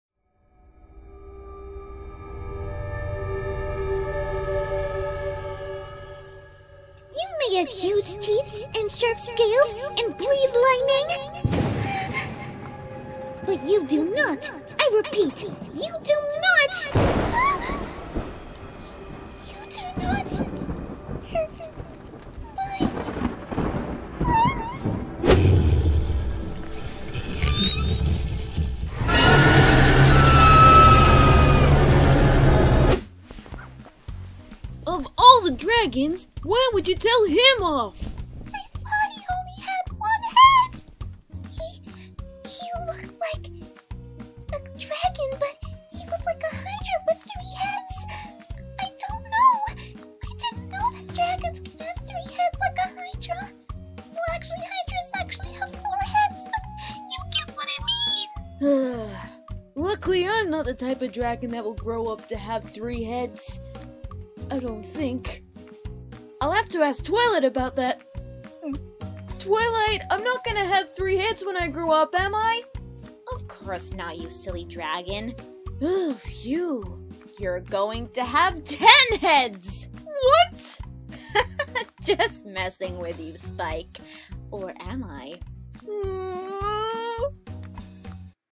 [MLP Comic Dub] Kaijushy (Spooky Comedy)
Goodness. What gave Fluttershy the courage to go up against a dragon THAT big?!

Wanna collab with me in a Comic dub? Email or message me if interested.